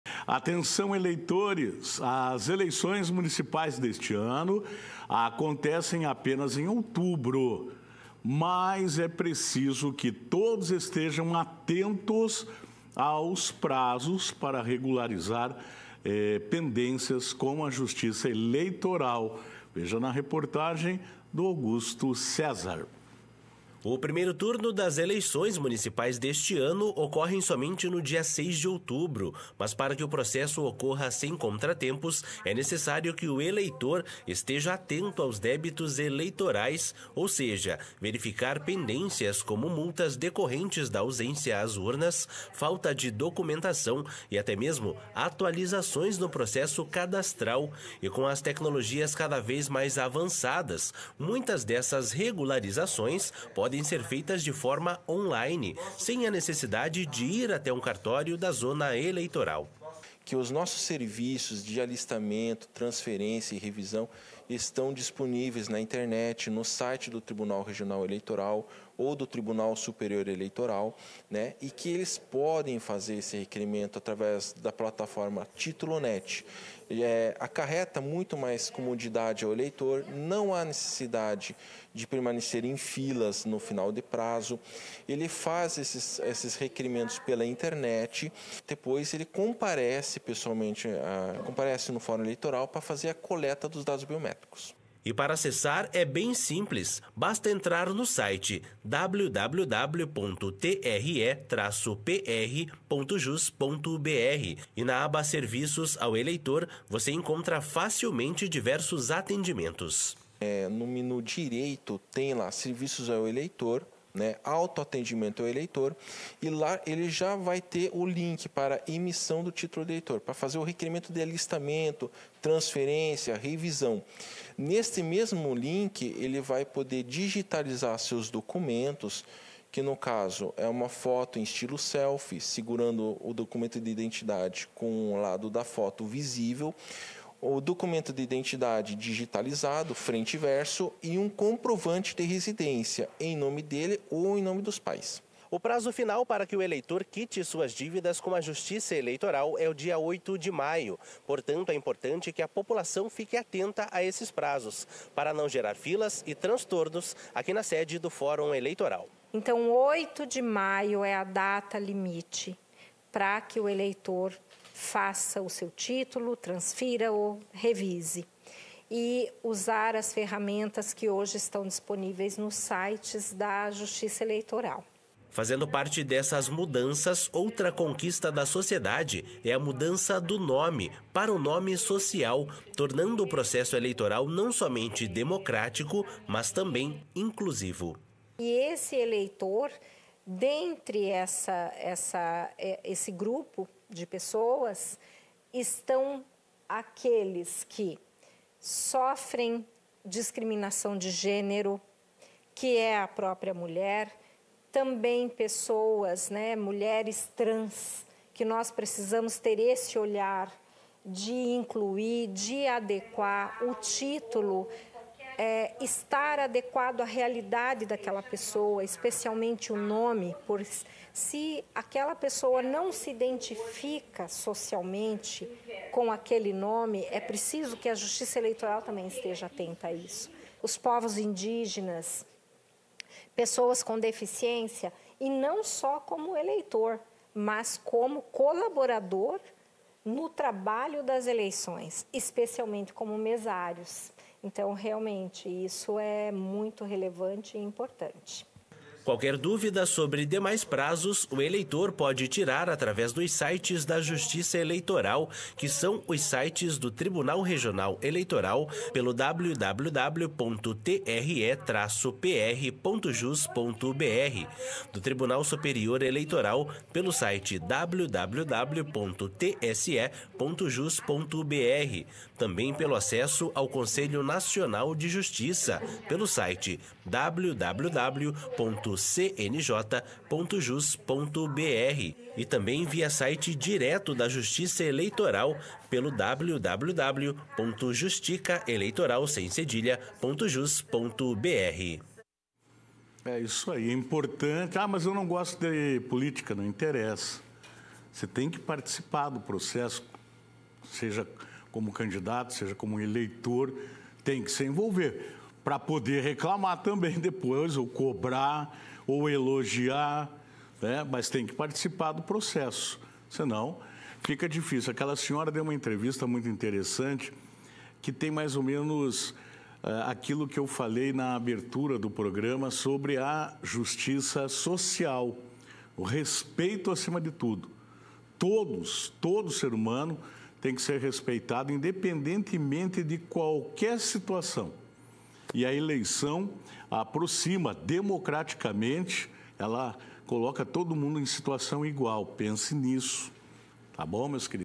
PENDÊNCIAS COM A JUSTIÇA ELEITORAL
ELEITORES EM DÍVIDA DEVEM RESPEITAR PRAZOS, SE QUISEREM VOTAR NAS ELEIÇÕES MUNICIPAIS